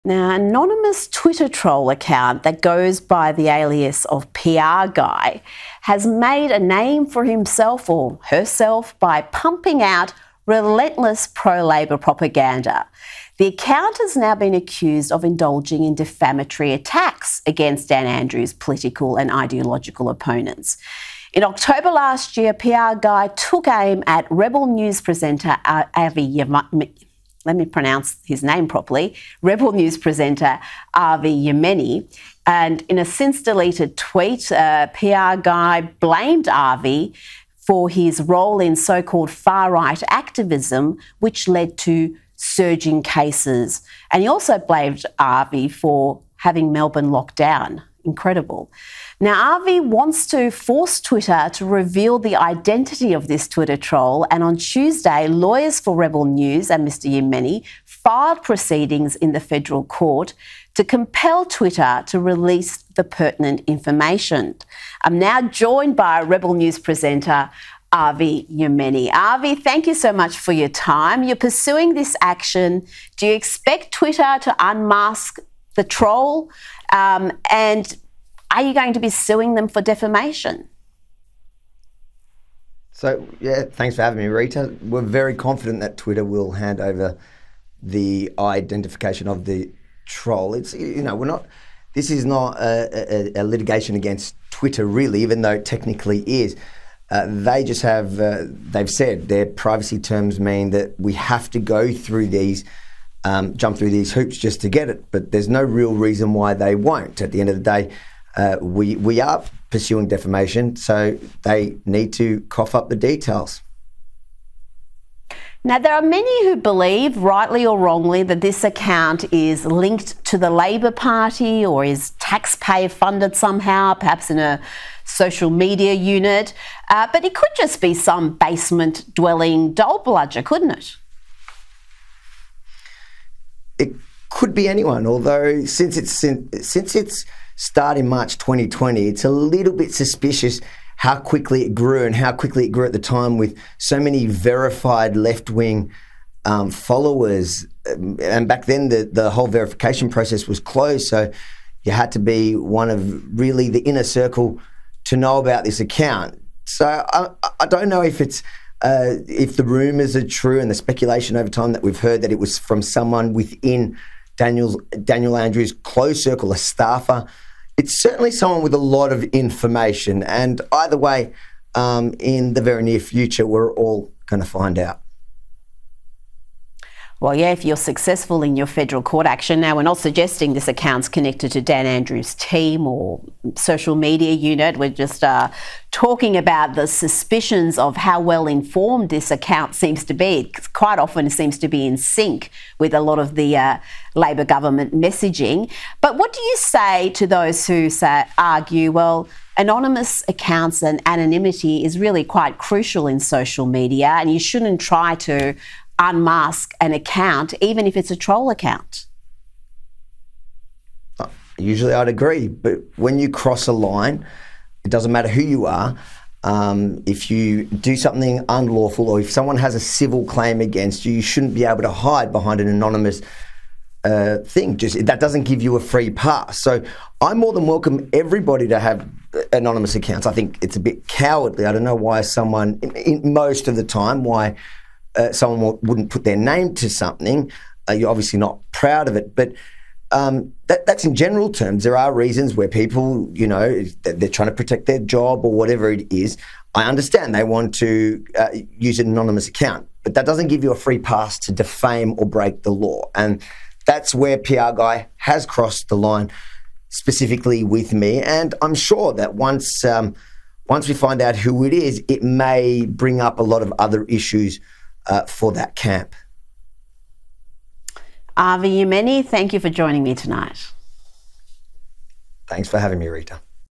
Time running out for Dan Andrews' anonymous Troll-in-Chief
PRGuy17 is set to be unmasked for my defamation proceedings.